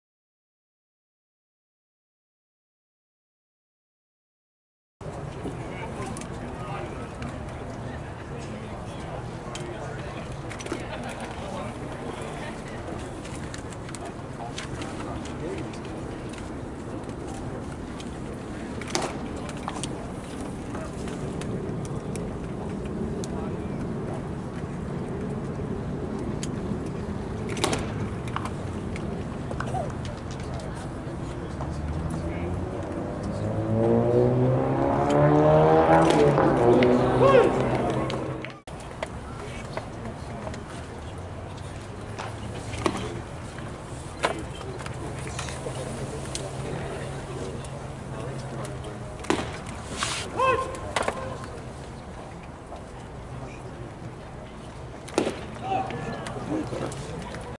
Corey Copping AFL
RHP Corey Copping of the Los Angeles Dodgers pitching for the Glendale Desert Dogs in the Arizona Fall League on 10/18/16.